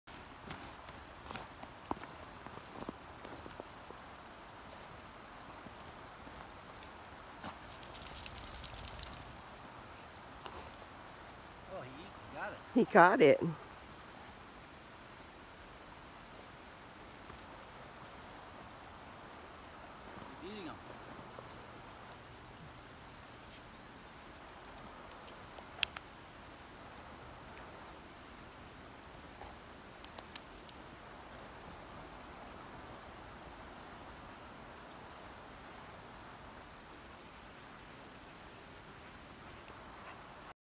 Old English Sheepdog catching snow balls
Boomer catching snow balls.🤗